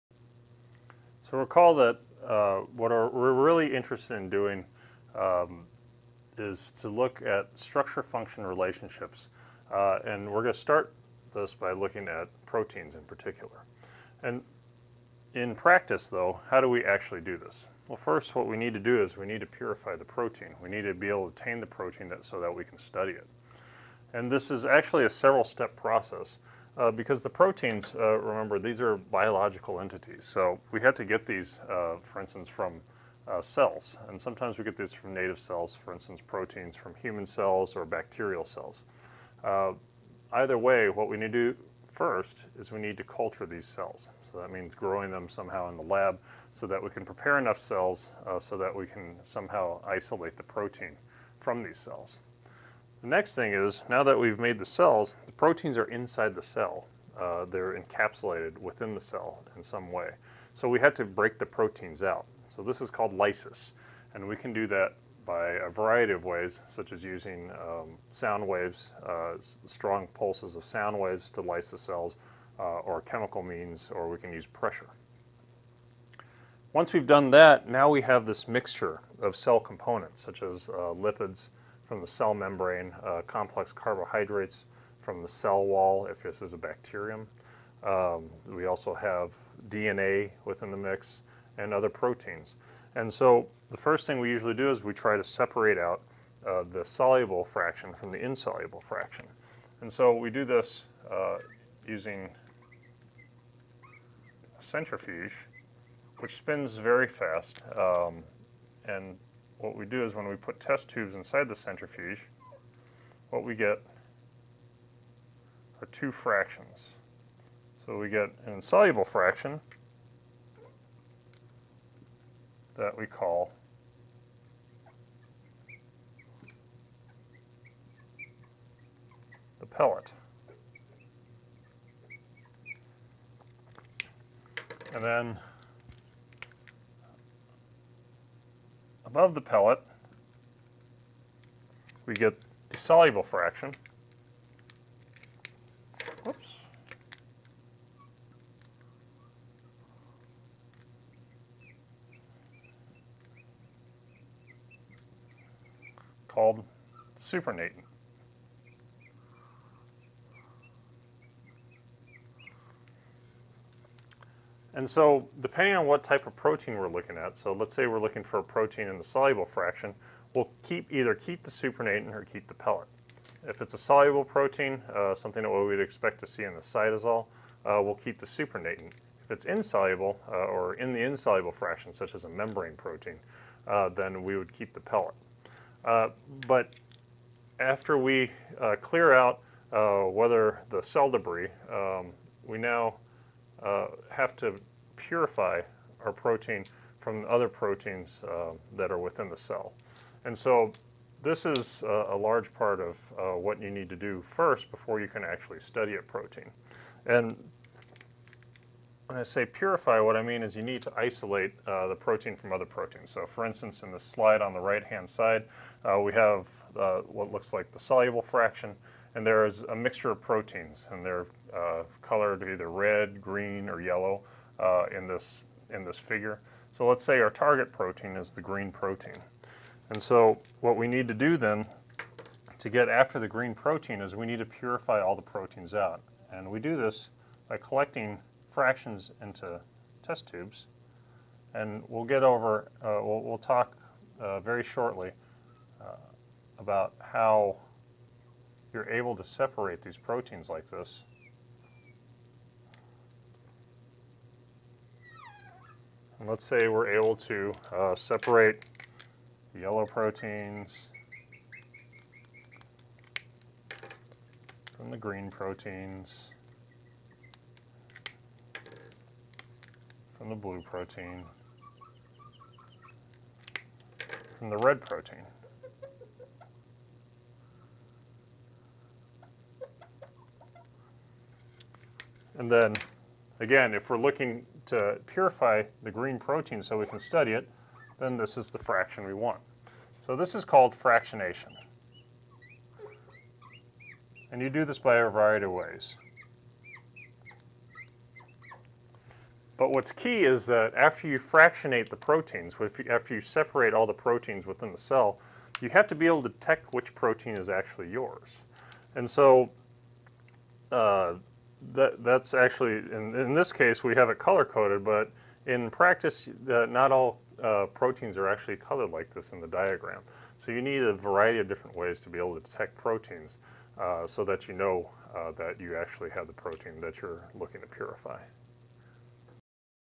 Topic 4.2 - Protein purification fractionation
Introduction to separating soluble proteins from a cell.